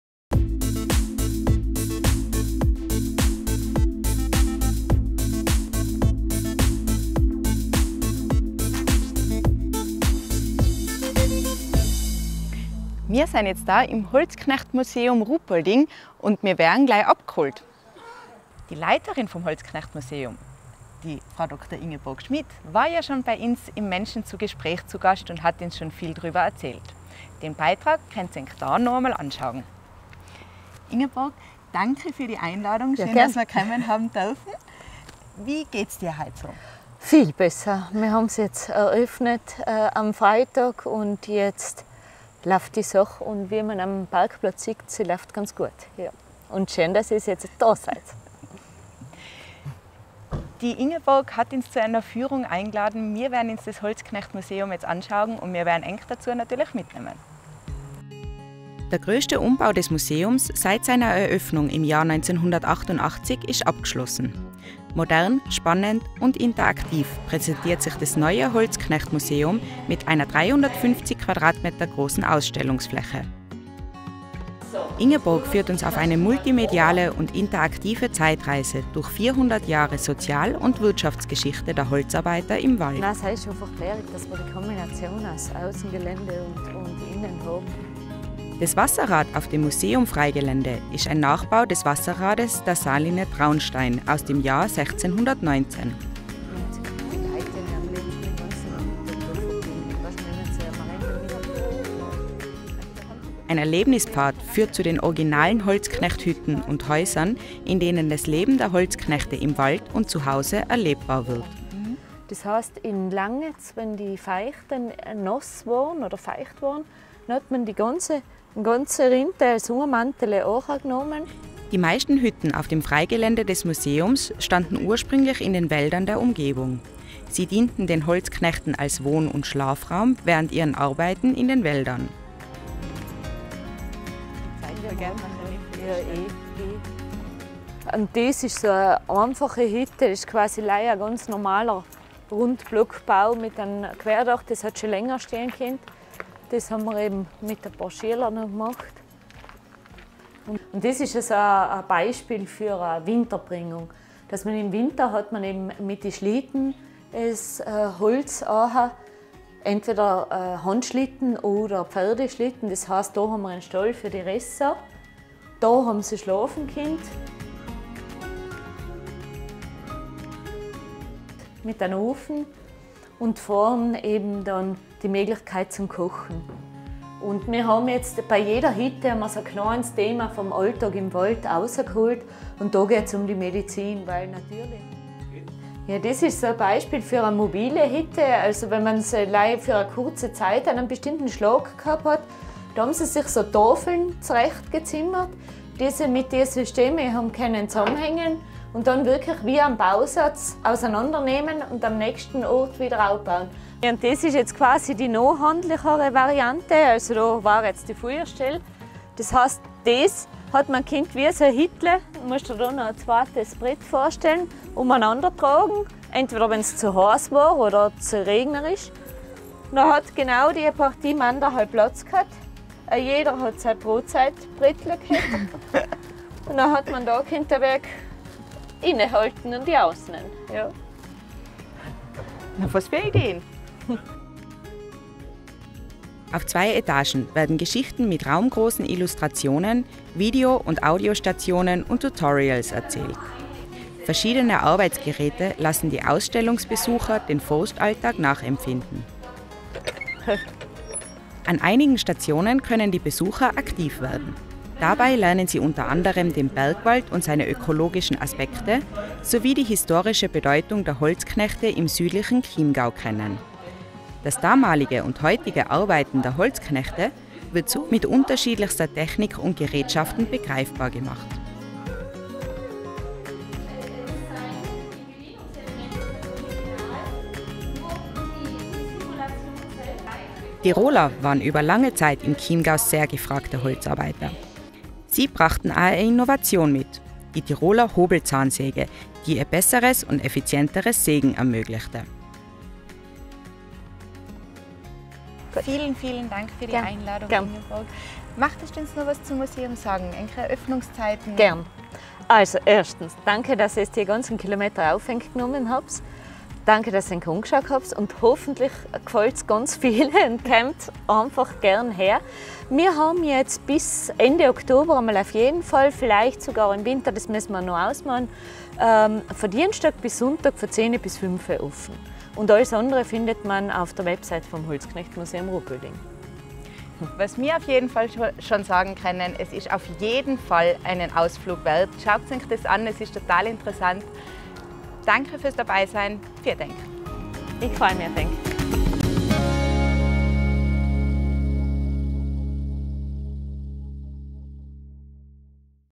Zu Besuch im Holzknechtmuseum Ruhpolding
Frau Dr. Ingeborg Schmid - sie hat Mieminger Wurzeln - ist Leiterin des Holzknechtmuseums Ruhpolding. Kurz nach der Neueröffnung des Museums durften wir die erste Führung durch die Welt der Waldarbeiter genießen.